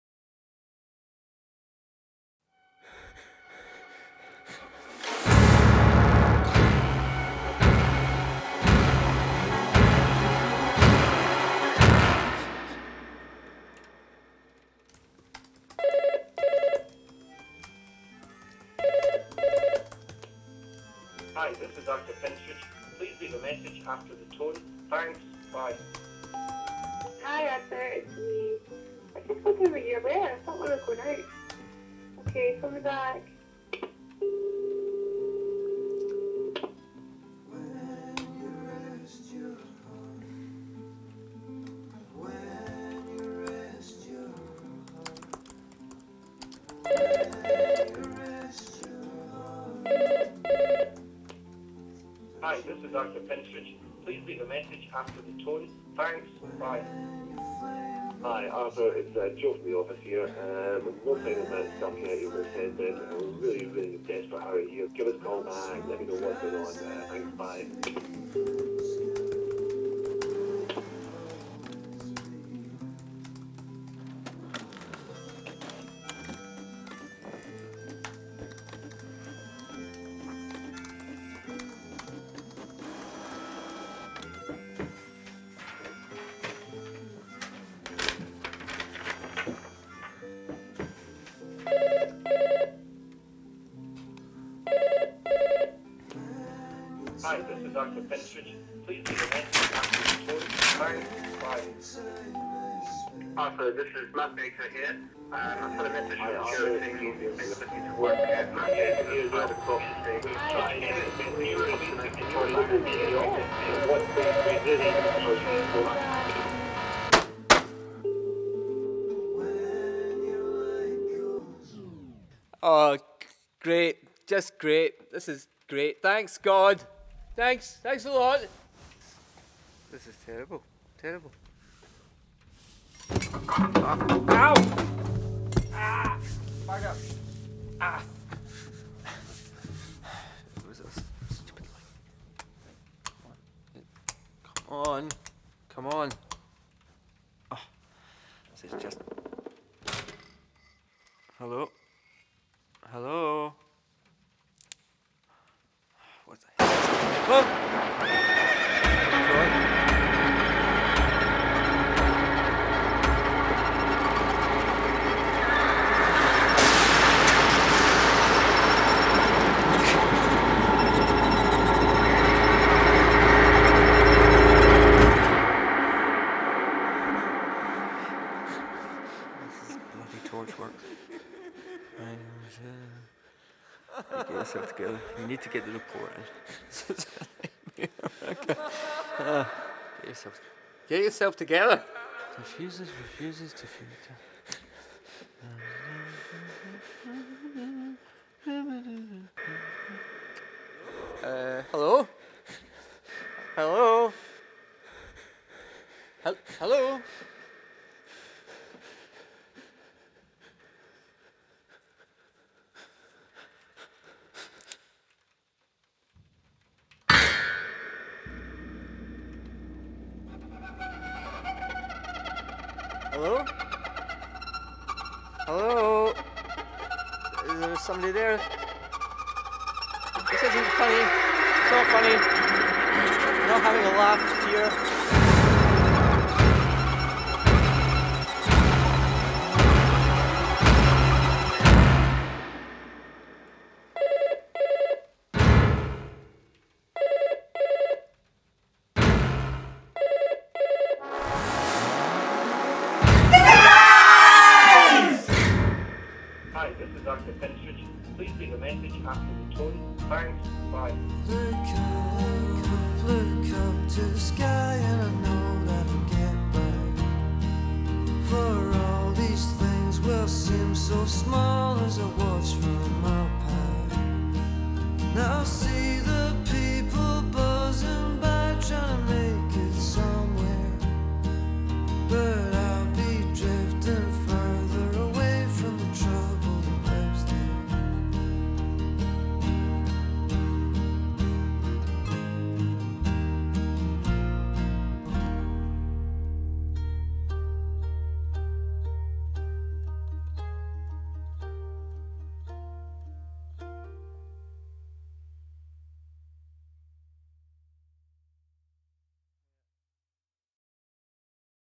Fused
A short horror comedy, shot on an iPhone, but with quite sophisticated sound design and music treatment.